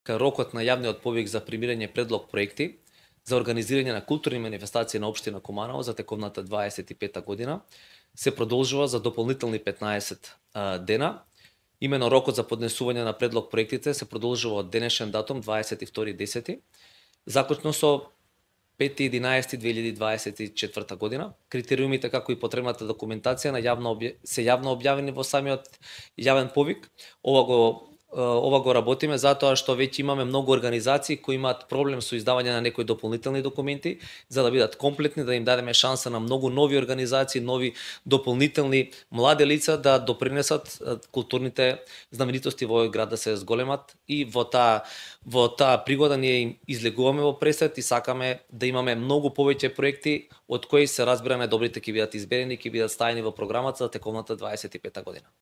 Продолжен рокот за прибирање предлог- проекти за културни манифестации
Продолжен е рокот за аплицирање на јавниот повик на Општина Куманово за предлог проекти за програмата за културни манифестации за претстојната 2025 година.